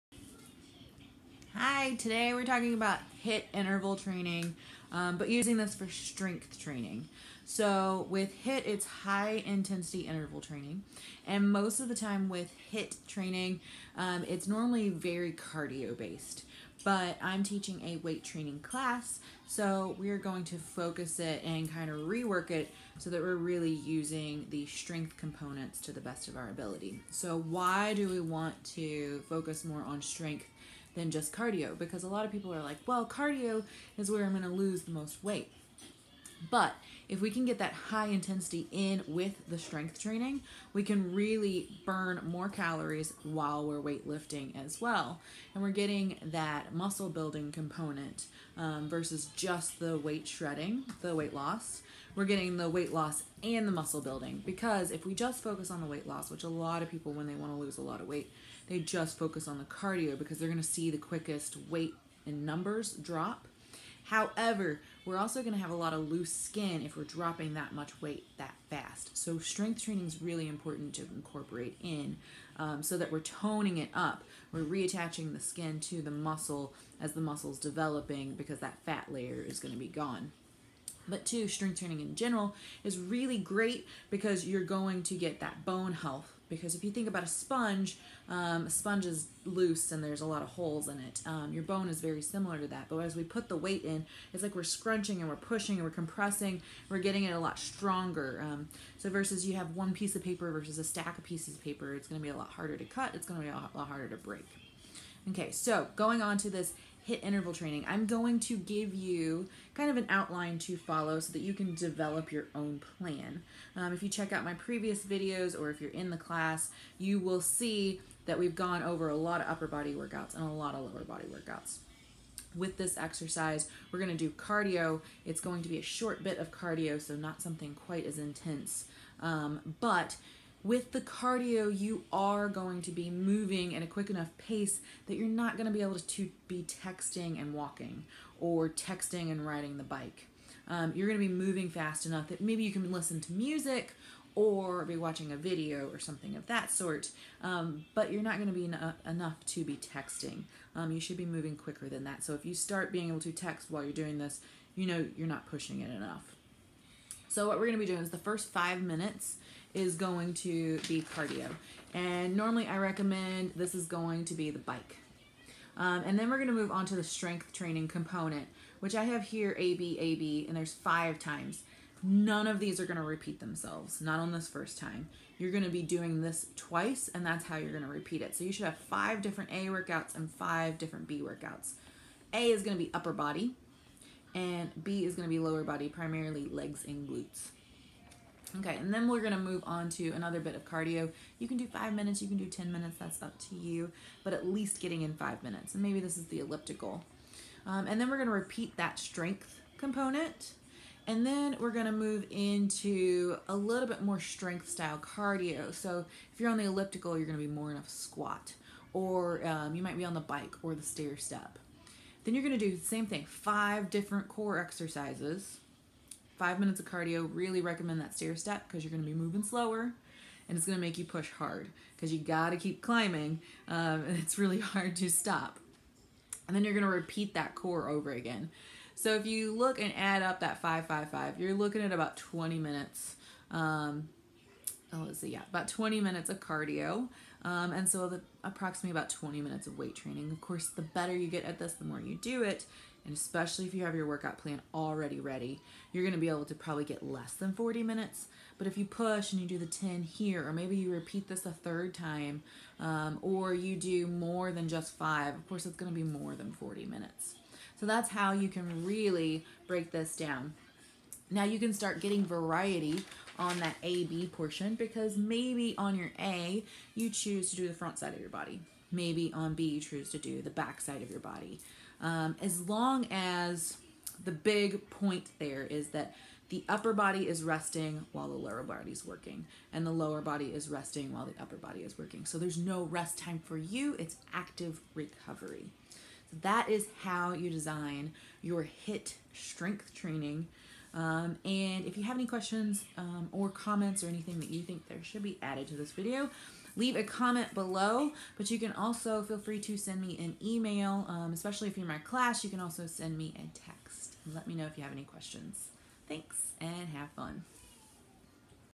HIIT Strength Training
Giving you the Why & How to design your HIIT strength training plan with this simple template.